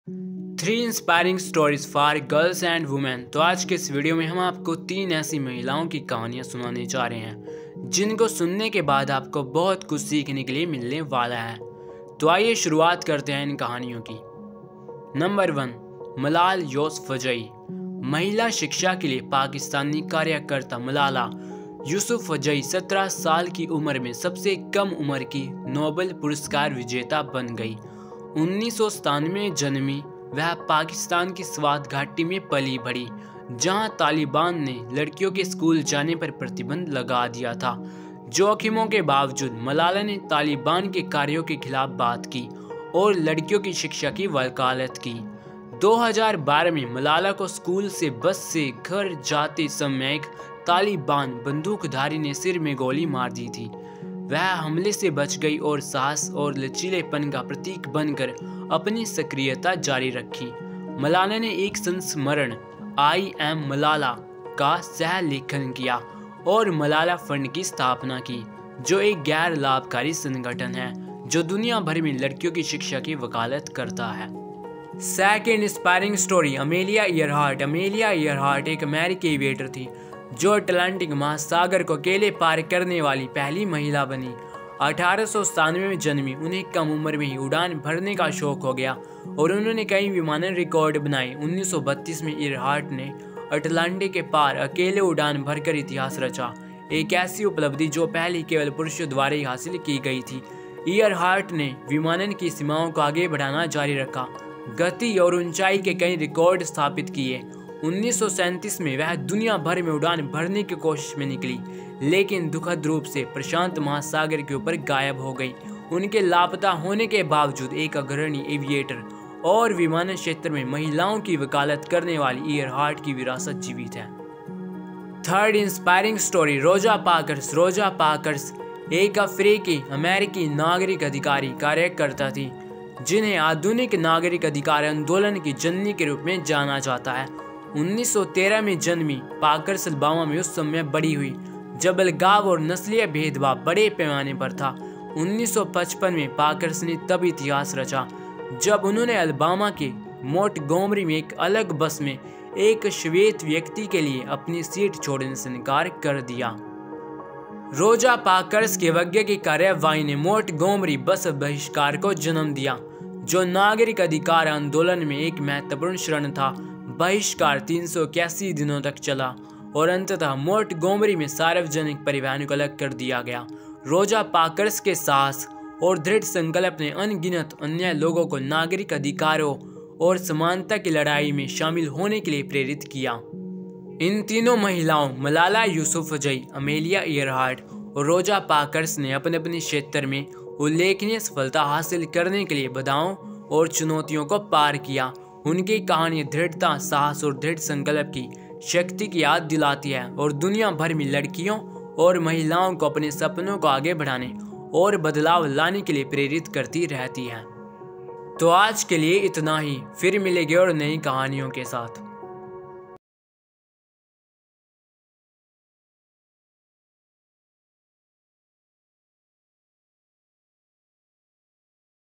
Three Inspiring Stories for Girls and Women | Inspirational story in Hindi | By Viren Radcliffe
Three Inspiring Stories for Girls and Women.
Malala Yousafzai
Malala Yousafzai, a Pakistani activist for female education, became the youngest Nobel Prize laureate at the age of 17. Born in 1997, she grew up in the Swat Valley in Pakistan, where the Taliban had banned girls from attending school. Despite the risks, Malala spoke out against the Taliban's actions and advocated for girls' education.
In 2012, Malala was shot in the head by a Taliban gunman while riding the bus home from school. She survived the attack and continued her activism, becoming a symbol of courage and resilience. Malala went on to co-author a memoir, "I Am Malala," and establish the Malala Fund, a non-profit organization that advocates for girls' education around the world.
Amelia Earhart.
Amelia Earhart was an American aviator who became the first woman to fly solo across the Atlantic Ocean. Born in 1897, she developed a passion for flying at a young age and went on to set many aviation records. In 1932, Earhart made history by flying solo across the Atlantic, a feat that had previously been accomplished only by men.
Earhart continued to push the boundaries of aviation, setting numerous speed and altitude records. In 1937, she embarked on an attempt to fly around the world, but tragically disappeared over the Pacific Ocean. Despite her disappearance, Earhart's legacy as a pioneering aviator and advocate for women in aviation lives on.
Rosa Parks.
Rosa Parks was an African American civil rights activist who became known as the "mother of the modern civil rights movement." Born in 1913, Parks grew up in Alabama at a time when segregation and racial discrimination were rampant. In 1955, Parks made history when she refused to give up her seat to a white man on a segregated bus in Montgomery, Alabama.
Parks' act of defiance sparked the Montgomery Bus Boycott, a pivotal moment in the civil rights movement. The boycott lasted for 381 days and eventually led to the desegregation of public transportation in Montgomery. Parks' courage and determination inspired countless others to join the fight for civil rights and equality.
These three women, Malala Yousafzai, Amelia Earhart, and Rosa Parks, each overcame obstacles and challenges to achieve remarkable success in their respective fields. Their stories serve as a reminder of the power of perseverance, courage, and determination, and continue to inspire girls and women around the world to pursue their dreams and make a difference.